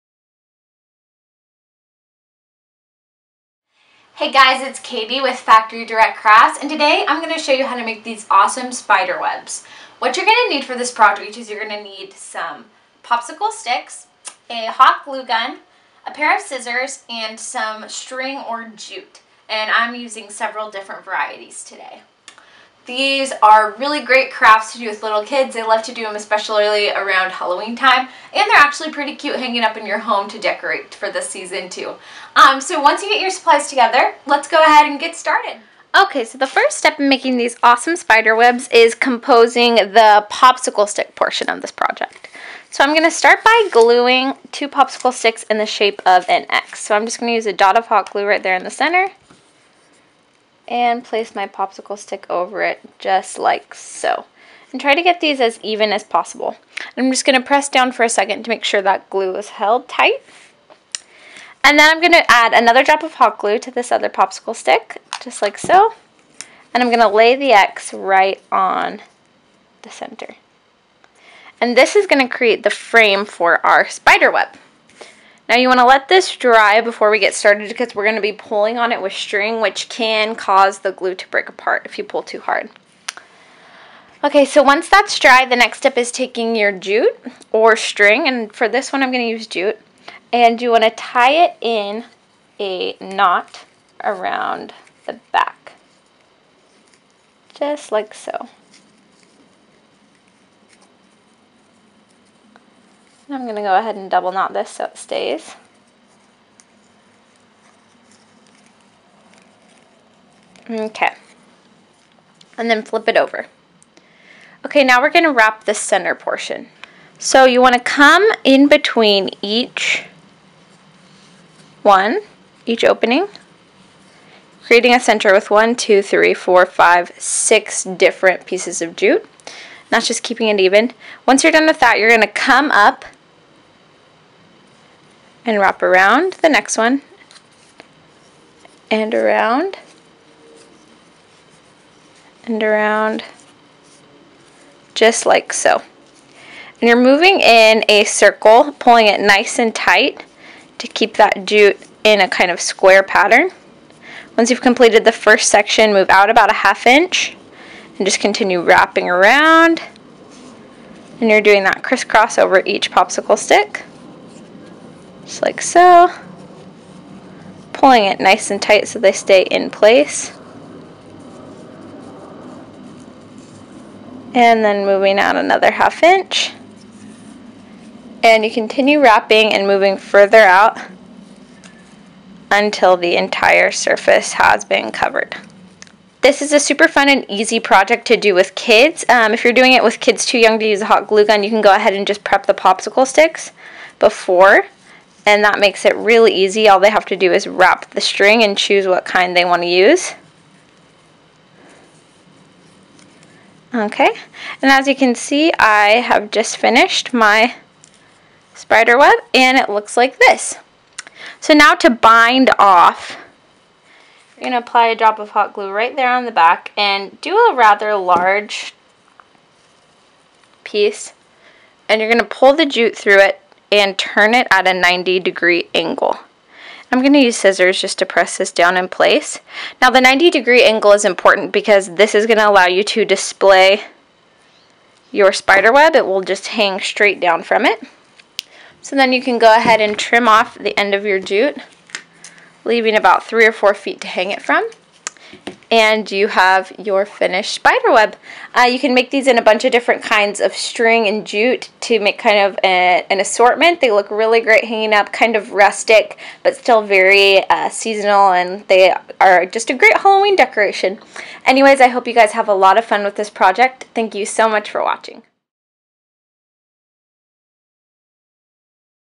Kid-Friendly Popsicle Stick and String Spider Webs
Let your spooky side show this Halloween with fun spider webs!